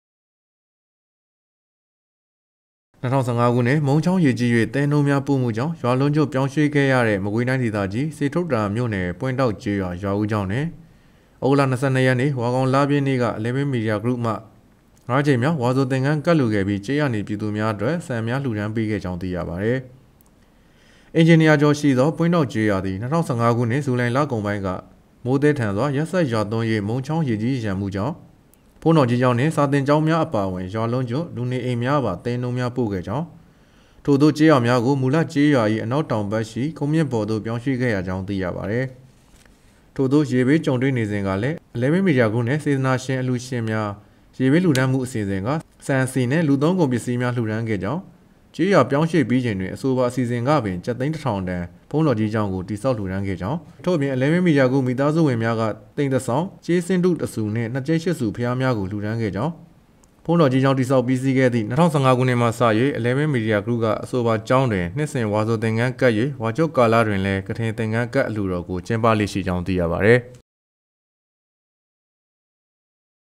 စေတုတ္တရာမြို့နယ် ရွာဦးကျောင်းတွင် EMG က ငါးကြိမ်မြောက် ဝါဆိုသင်္ကန်း ဆက်ကပ်လှူဒါန်း
၂၀၁၅ ခုနှစ်က မုန်းချောင်း ရေကြီး၍ သဲနုန်းများ ပို့ချမှုကြောင့် ရွာလုံးကျွတ် ပြောင်းရွှေ့ခဲ့ရသည့် စေတုတ္တရာမြို့နယ် ပွင့်တောက်ကျေးရွာ ရွာဦးကျောင်းတွင် ELEVEN MEDIA GROUP က ငါးကြိမ်မြောက် ဝါဆိုသင်္ကန်း ဆက်ကပ်လှူဒါန်းခဲ့ပြီး ကျေးရွာနေ ပြည်သူများအတွက် ဆန်များ လှူဒါန်း (ရုပ်သံ)

၂၀၁၅ ခုနှစ်က မုန်းချောင်း ရေကြီး၍ သဲနုန်းများပို့မှုကြောင့် ရွာလုံးကျွတ် ပြောင်းရွှေ့ခဲ့ရသည့် မကွေးတိုင်းဒေသကြီး စေတုတ္တရာမြို့နယ် ပွင့်တောက်ကျေးရွာ ရွာဦးကျောင်းတွင် ဩဂုတ် ၂၂ ရက် (ဝါခေါင်လပြည့်နေ့) က Eleven Media Group မှ ငါးကြိမ်မြောက် ဝါဆိုသင်္ကန်း ကပ်လှူခဲ့ပြီး ကျေးရွာနေပြည်သူများအတွက် ဆန်များလှူဒါန်းပေးခဲ့ကြောင်း သိရပါတယ်၊